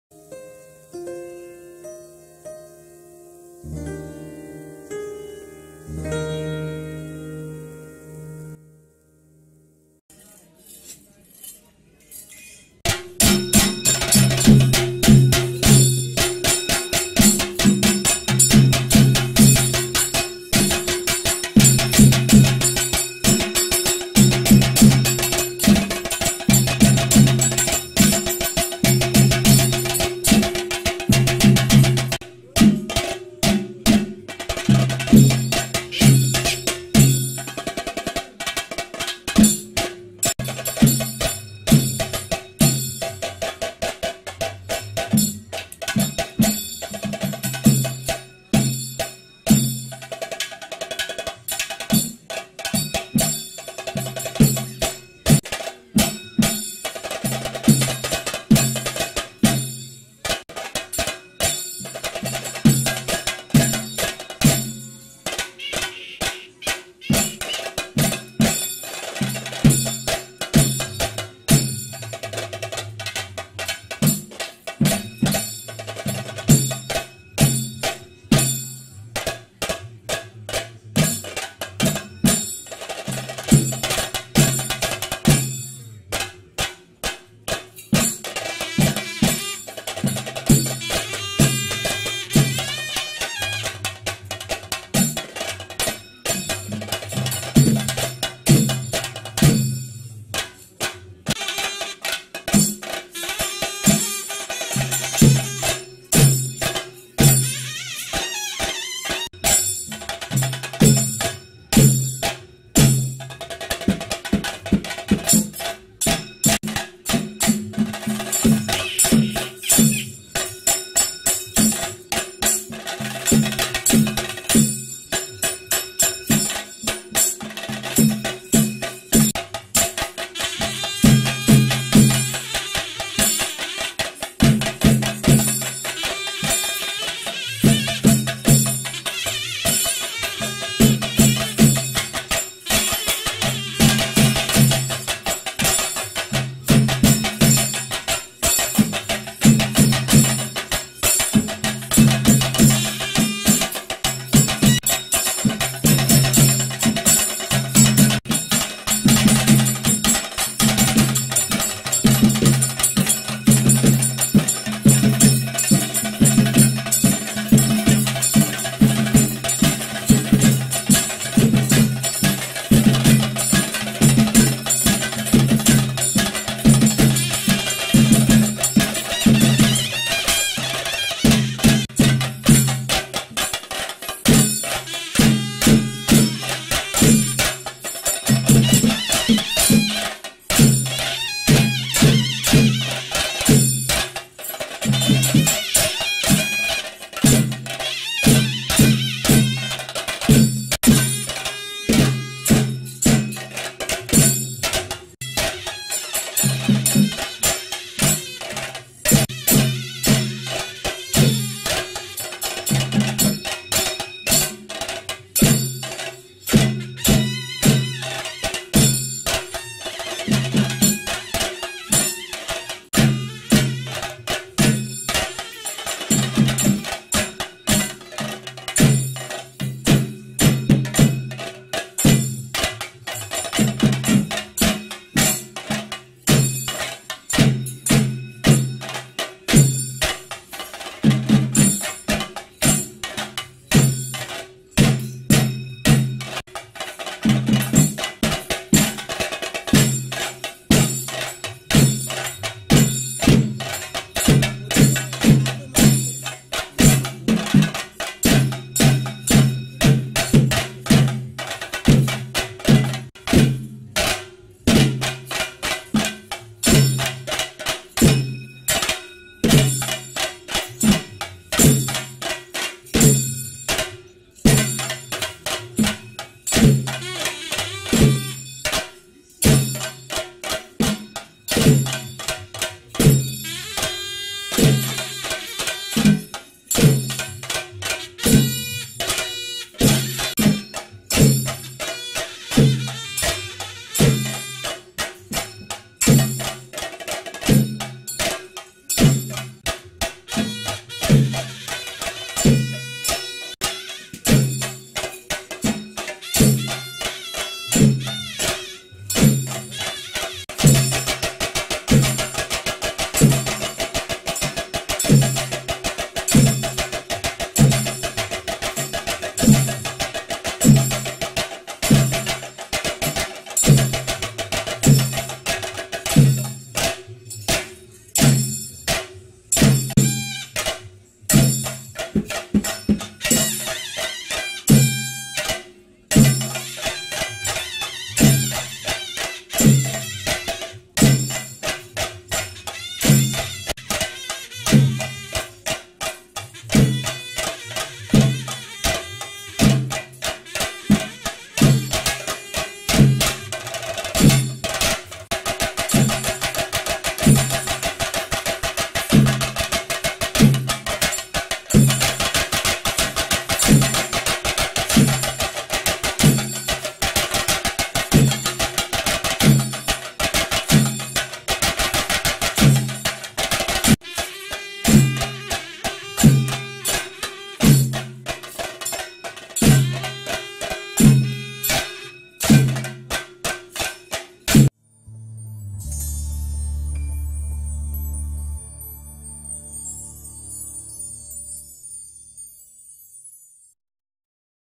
theyyam | മുത്തപ്പൻ തിറ | thiruvappana | part 1
thiruvappana pallivetta video taken from koothuparamba muthappan kshethram 2018.